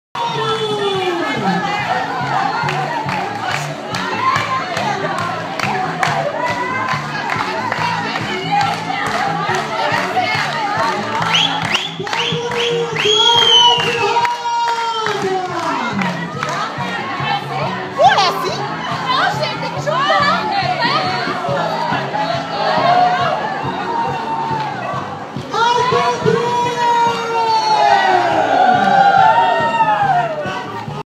Professores de Cabo Frio fazem quadrilha na frente da Prefeitura como forma de protesto - 1
Os professores de Cabo Frio ligados ao Sindicato dos Profissionais da Educação (Sepe Lagos), fizeram um protesto bem-humorado na frente da Prefeitura na tarde dessa sexta-feira (01/07/2016)